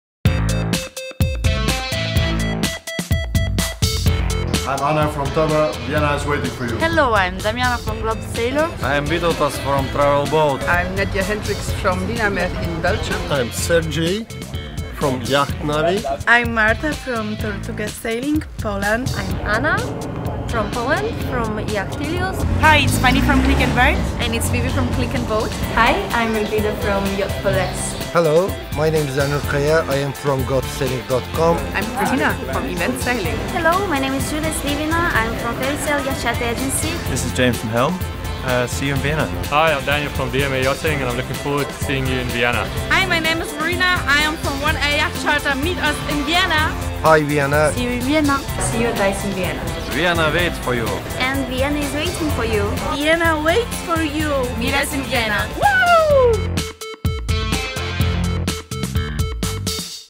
Vienna waits for you - ICE'19
Short movie filmed during Charter Meetup Athens 2019. Companies featured in the video: Tubber, GlobeSailor, Travel Boat, Dynamer World Wide Yachting, Tortuga Sailing, Yachtilius, Click&Boat - Yacht Charter, Yacht4Less, Yacht Charters & Sailing, GotoSailing.com, ivent-sailing, FairySail, Helm, DMA Yachting, 1a Yachtcharter & Yacht Navi